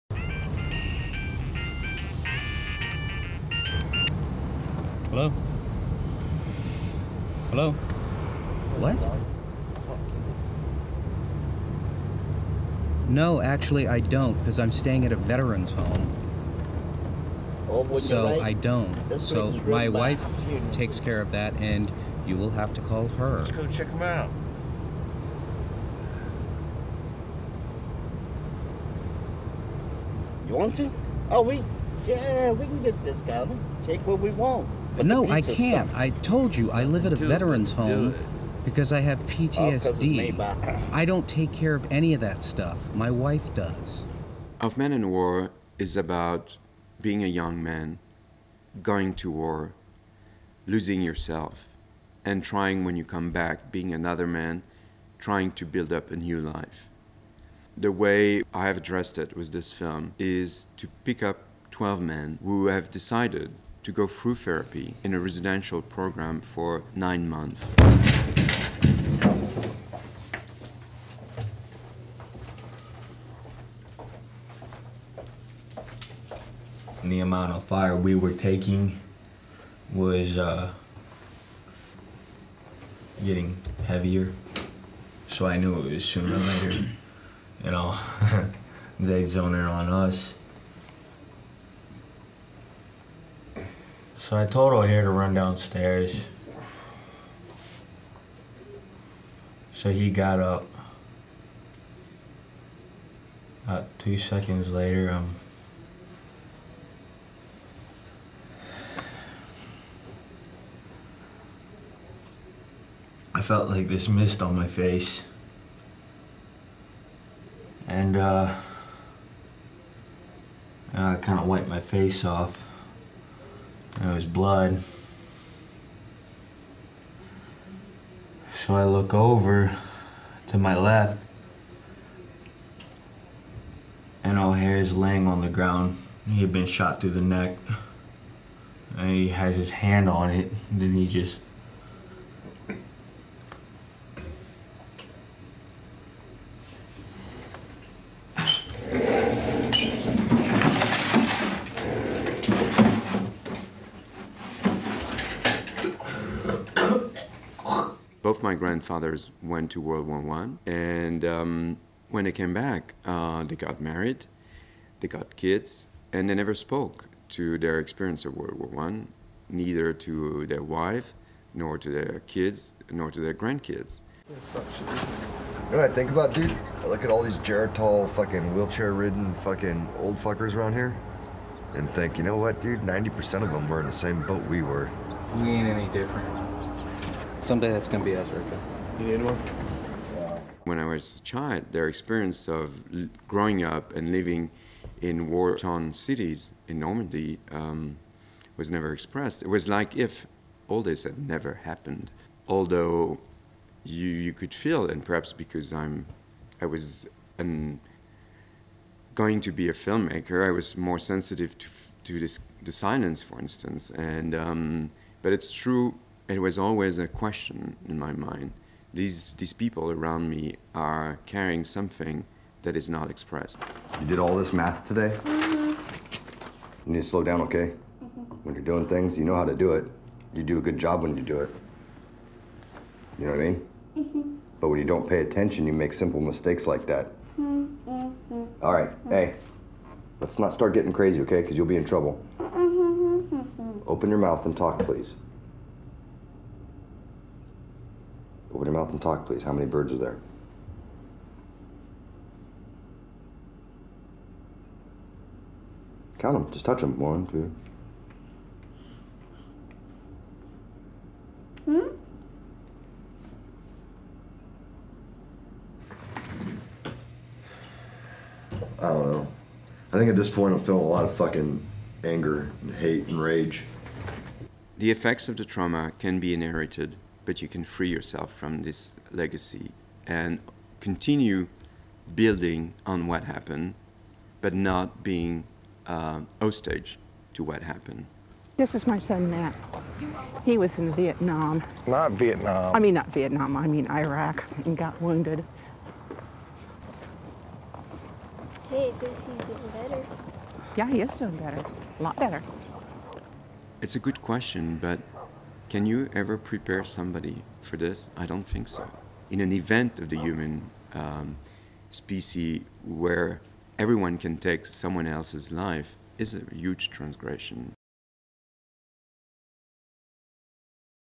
IDFA 2014 | Report | Laurent Bécue-Renard on Of Men and War
Director Laurent Bécue-Renard discusses his film Of Men and War, winner of the VPRO IDFA Award for Best Feature-Length Documentary.